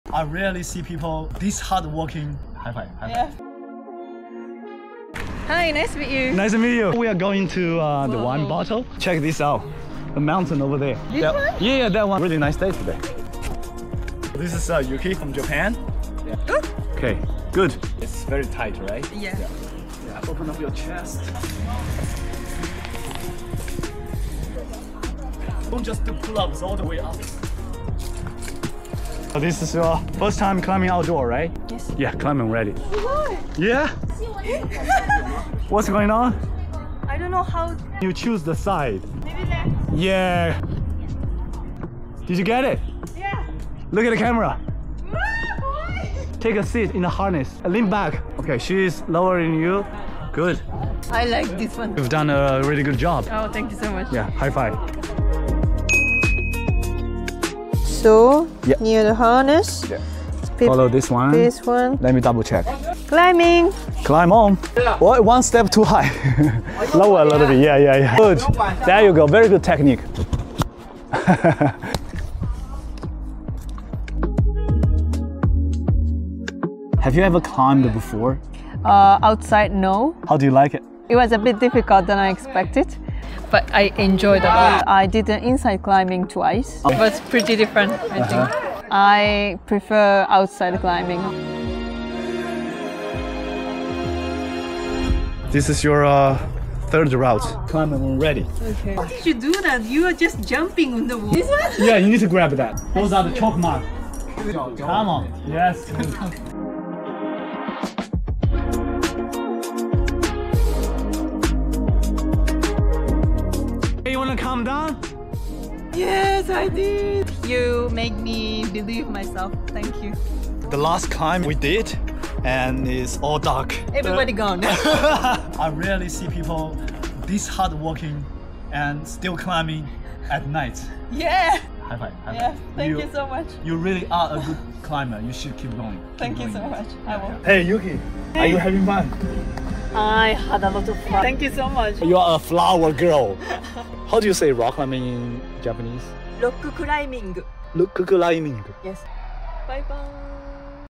Inside a rock climbing session at Wine Bottle Crag in Yangshuo
Wine Bottle Crag is one of the most visited crags in Yangshuo, many beginner climbers start their outdoor climbing journey from there, intermediate climbers also find their joys there.

In this vlog, I guided a Japanese girl who tried indoor climbing twice, but never did any outdoor climbing, let's find out what she has to say about outdoor climbing in Yangshuo.

There's a lot of difference in between indoor climbing and outdoor climbing, in my opion, I think it's better to start with outdoor climbing, but if the city where you from doesn't have any crag, then start climbing from indoor, however, you will need to transition from indoor to outdoor, and you will also need to climb both indoor and outdoor to progress when you hit plateau.

室内的人工岩壁和户外的野攀，差别挺大的，建议新手如果有条件，最好从户外野攀入门， 如果生活的城市没有野攀场地，只能室内开始，但是从室内过渡到野攀还是需要一个过程的。而且如果想要突破瓶颈期，室内和野攀需要双修才行。阳朔酒瓶山岩场非常适合新手体验或者是从室内攀岩过渡到户外野攀的初学者。

Need a outdoor climbing guide in Yangshuo?

Sam's Wechat ID: yangshuobnb